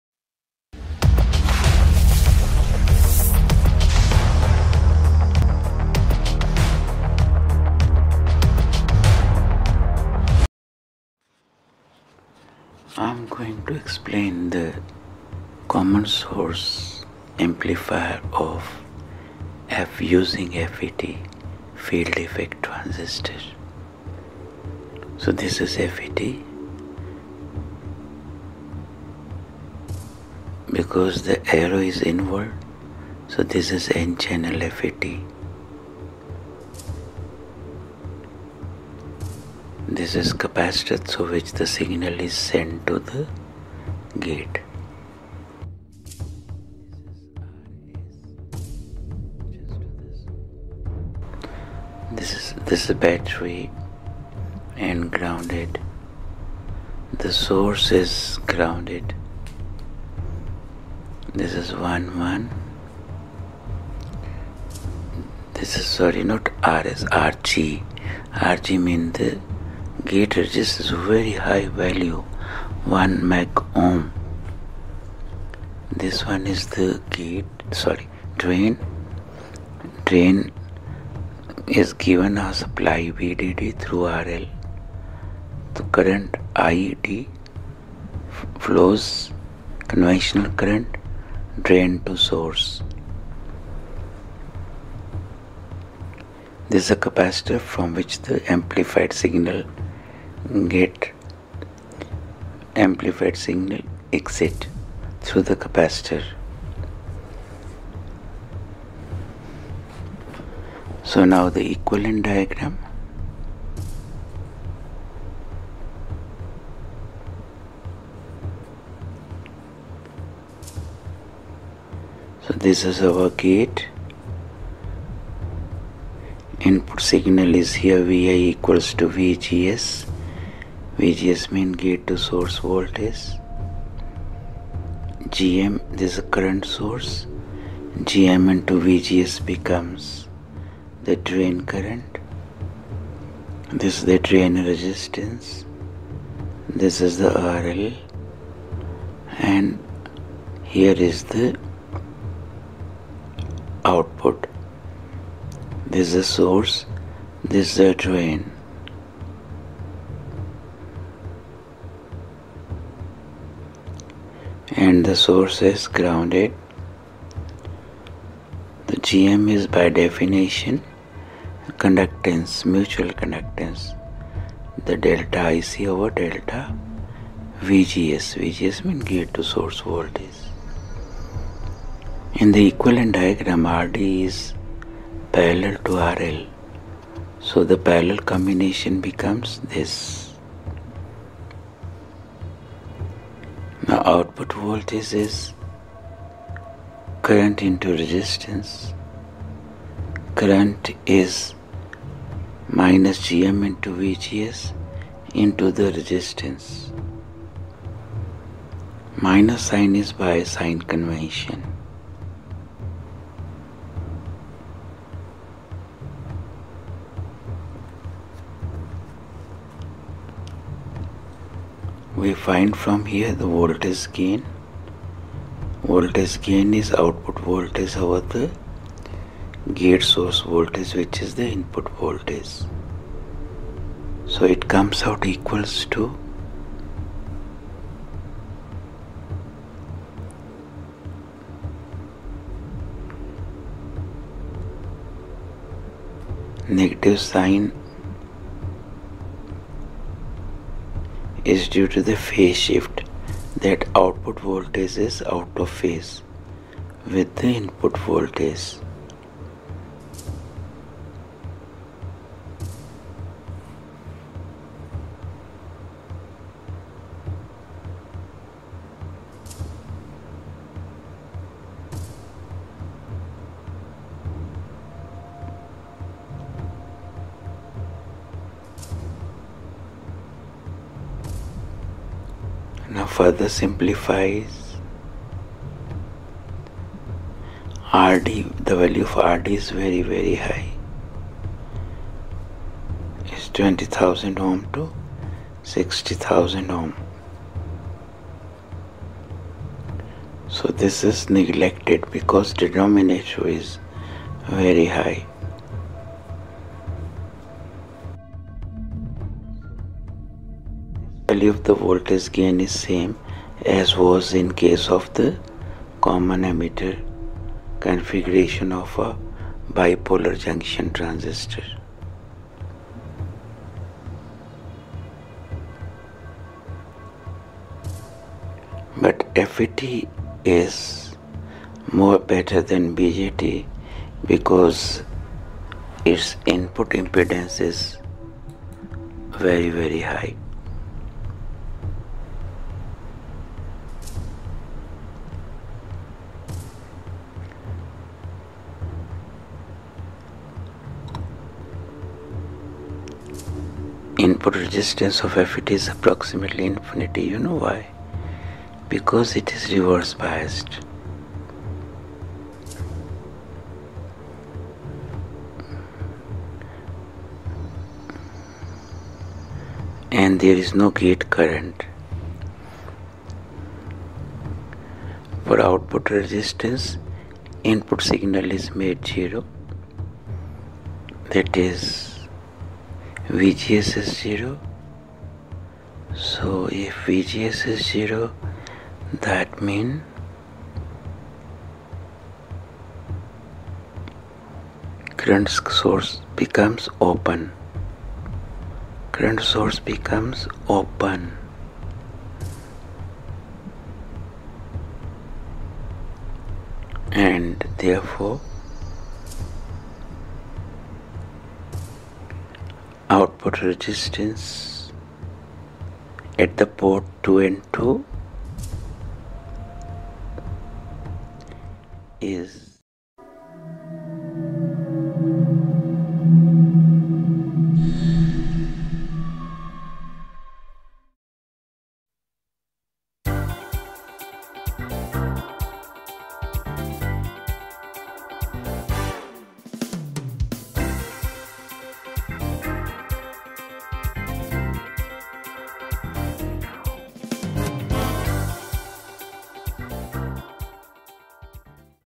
FET as an Amplifier: Common Source
Small signal amplifiers can also be made using Field Effect Transistors.
FET have the advantage over BJT of having an extremely high input impedance along with a low noise output making them ideal for use in amplifier circuits that have very small input signals. You can find handwritten notes on my website in the form of assignments.